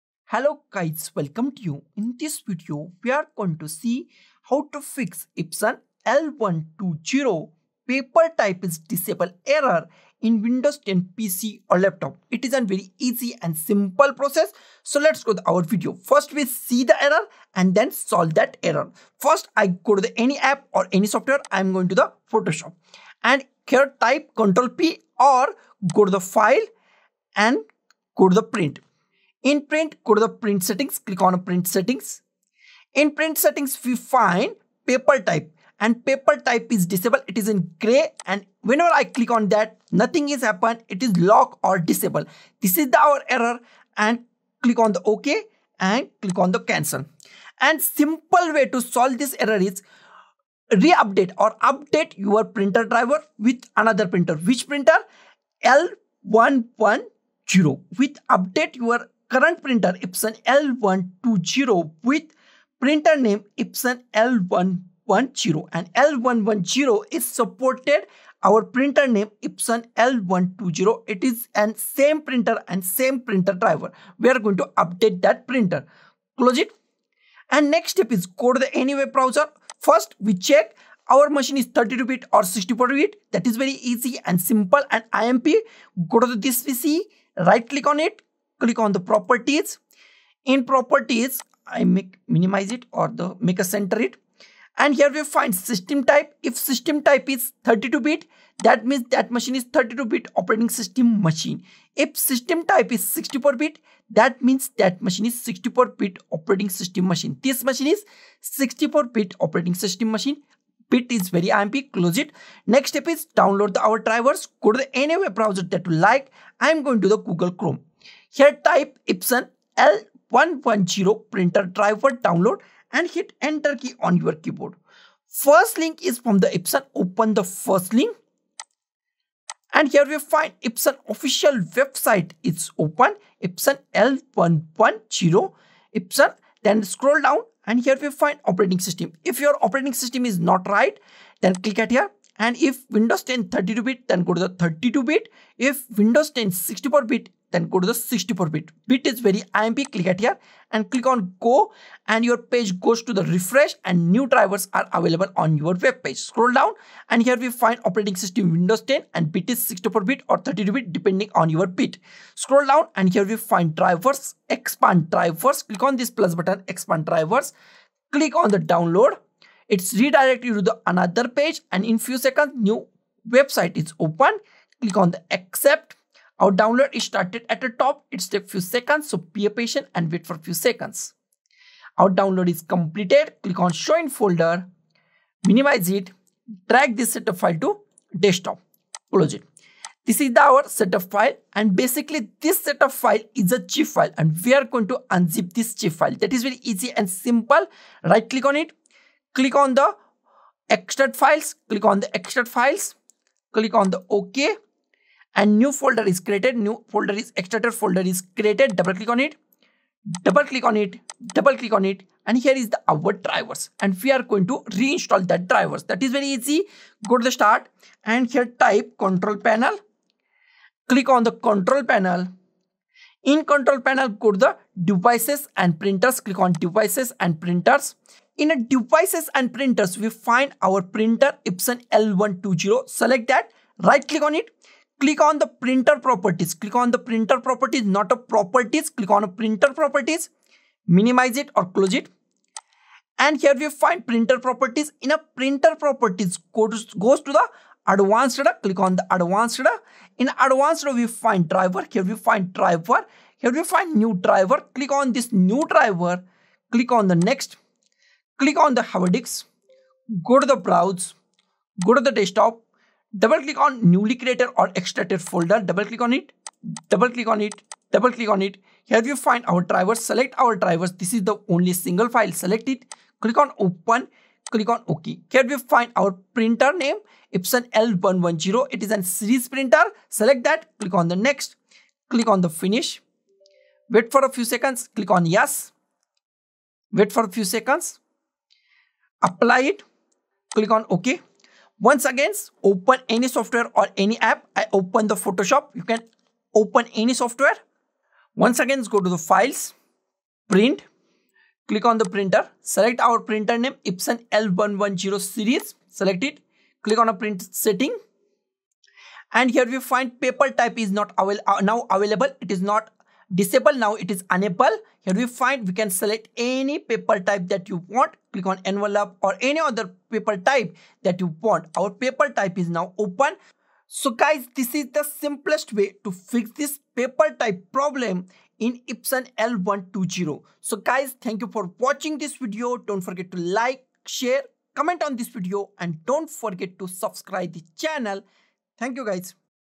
How to Fix Epson L120 Printer Paper Type is Disabled Error in Windows 10 PC or Laptop
Are you facing the "Paper Type is Disabled" error on your Epson L120 printer in Windows 10? In this video, we'll guide you through the steps to resolve this issue and get your printer back to working smoothly. Follow our detailed instructions to fix the problem quickly and efficiently.

In this video, you will learn:
Common causes of the Epson L120 "Paper Type is Disabled" error.
How to update your Epson L120 printer drivers.
How to adjust printer settings to re-enable paper type selection.
Troubleshooting tips for resolving the error.

Timestamps:
0:00 - Introduction
0:15 - Check Epson L120 Paper Type is Disabled Error
0:47 - How to Enable Epson L120 Paper Type
1:13 - Check windows 10 is 32 bit or 64 bit
1:52 - Downloading Epson L110 Printer Driver from the Epson Website
3:09 - Extracting Downloaded Files
3:36 - Updating Epson L120 printer drivers with Epson L110 printer drivers
4:51 - Check Epson L120 Paper Type is working or Not
5:23 - Conclusion